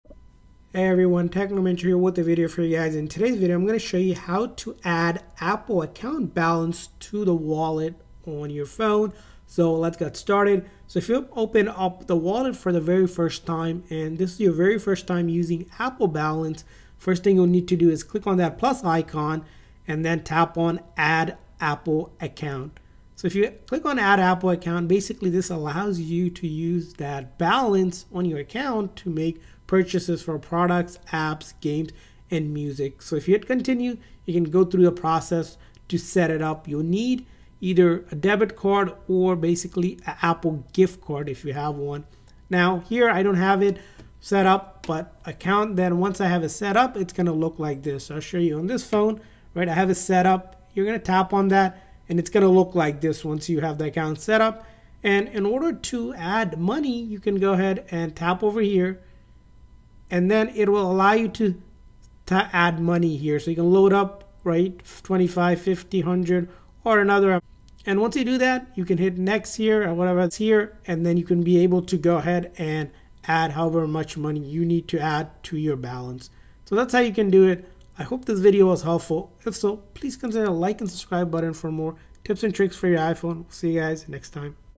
How To Add Apple Account Balance To Wallet?
Here's how to add Apple account balance to Wallet app on iPhone!